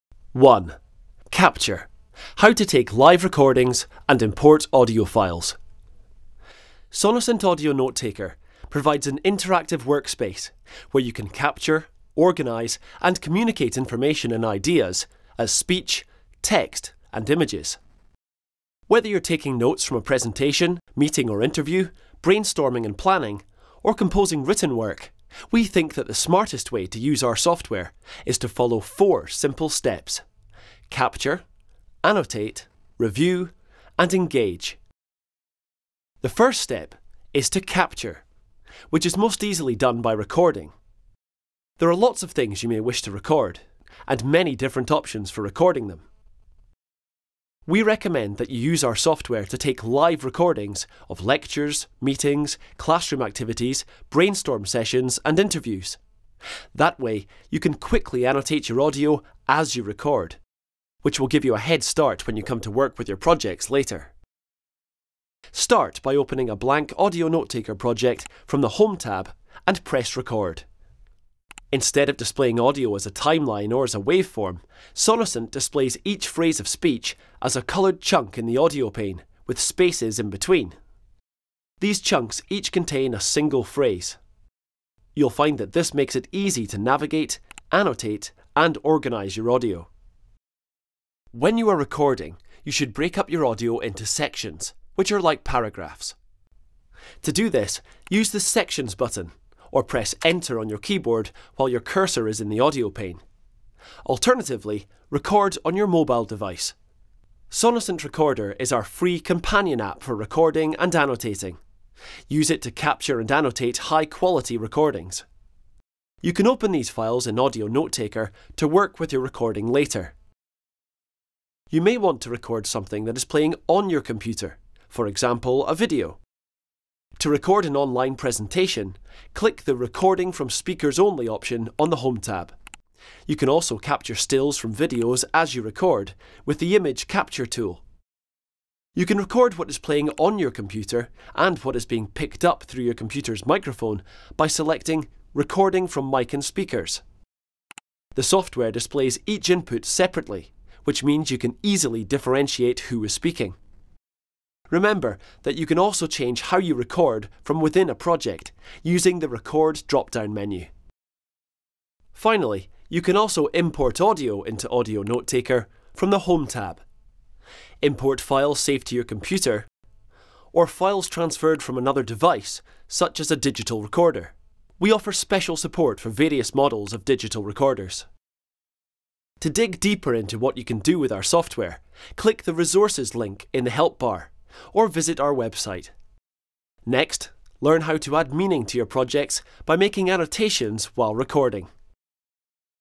Using Sonocent Audio Notetaker: 1. Capture
The first in our series of 4 videos explaining how to work with our award-winning study and productivity tool.

Sonocent Audio Notetaker provides an interactive workspace where you can capture, organise and communicate information and ideas - as speech, text and images.

Whether you are taking notes from a presentation, meeting or interview; brainstorming and planning; or composing written work, we think that the smartest way to use our software is to follow four simple steps: Capture, Annotate, Review and Engage.

The first step is to capture - which is most easily done by recording. There are lots of things you may wish to record, and many different options for recording them.